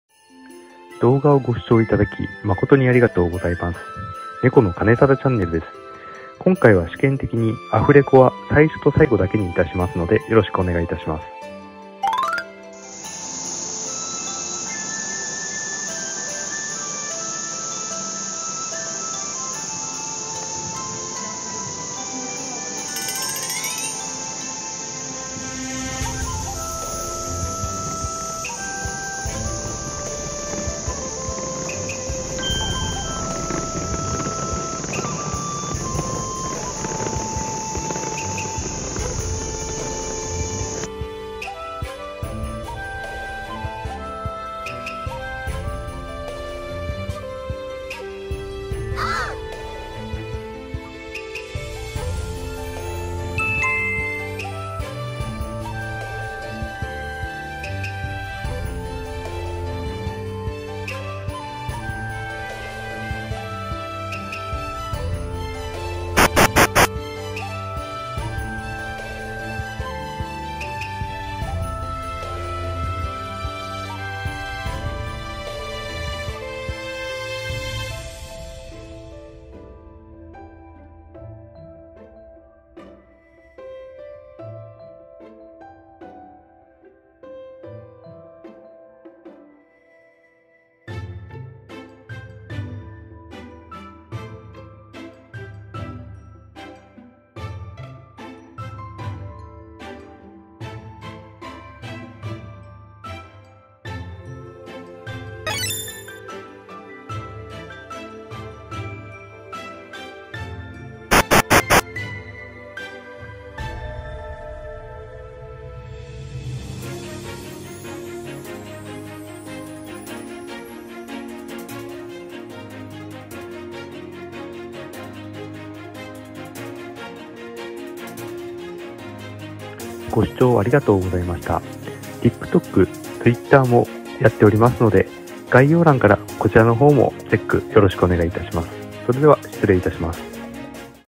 靖国八千代食堂利用の動画
靖国神社参拝動画の続きになります。
靖国神社参拝の動画はこちらから

TwitterとTikTok やってますのでこちらもよろしくお願いします。